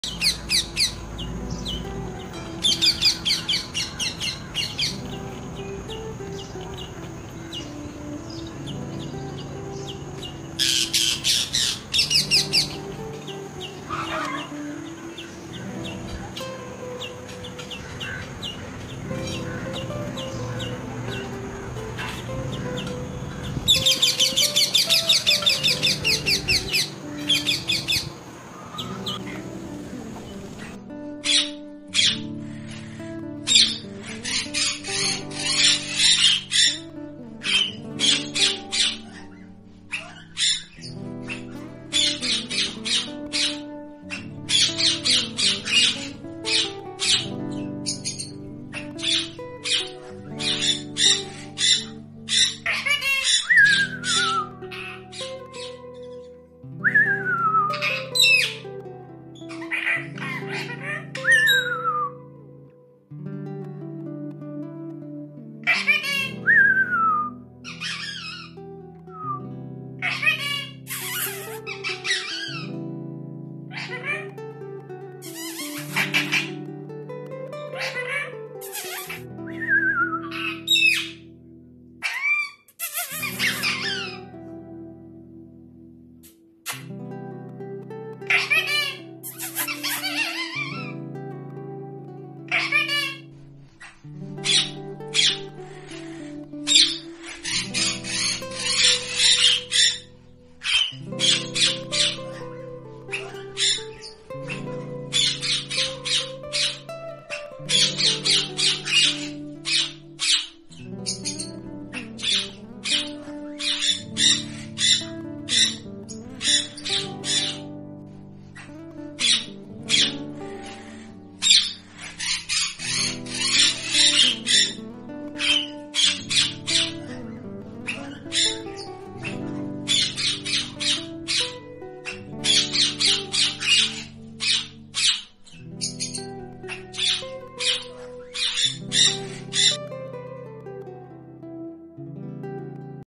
Indian Ringneck Parrots 🦜 in the nature and talking in smart away.
It's  a wonderful video of  lovely Indian Ringneck Parrots.